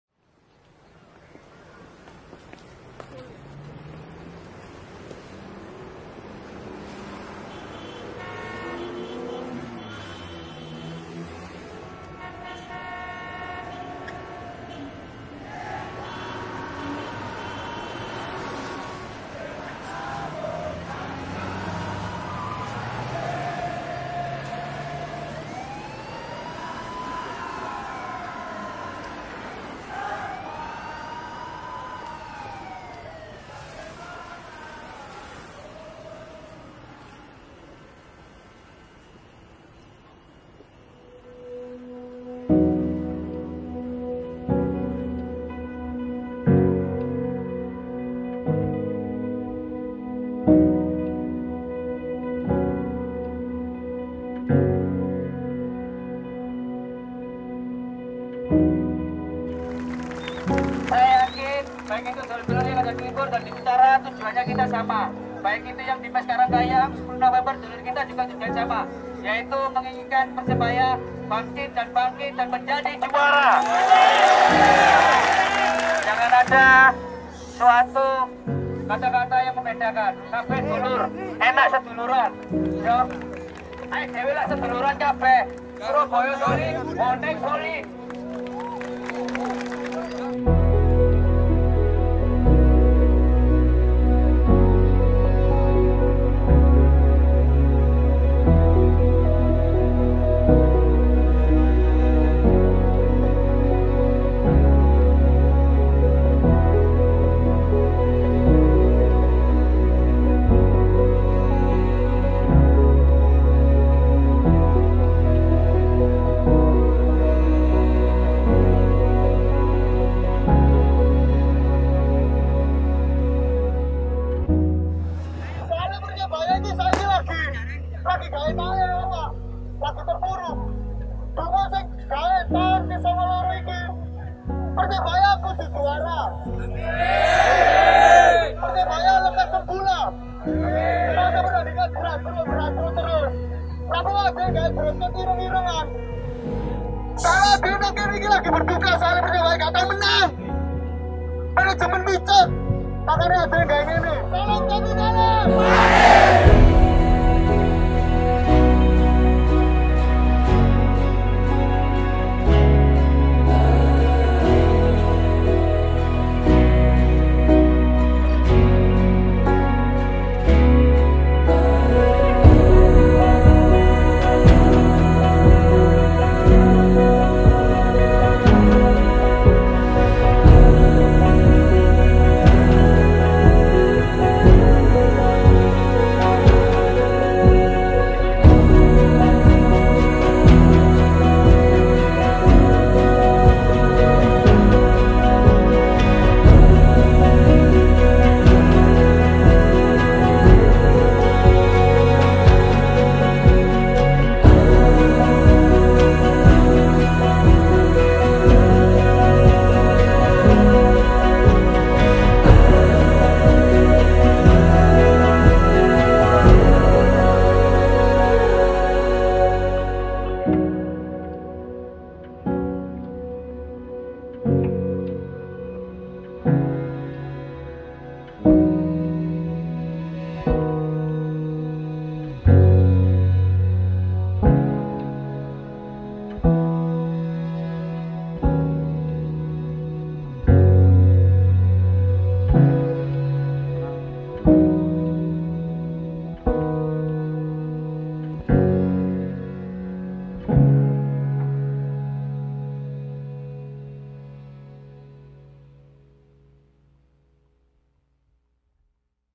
ANNIVERSARY 92TH 'LEKAS SEMBUH PERSEBAYAKU'
Banyak doa telah kita panjatkan, salah satu doa untukmu kebanggaan. Lekas sembuh Persebayaku.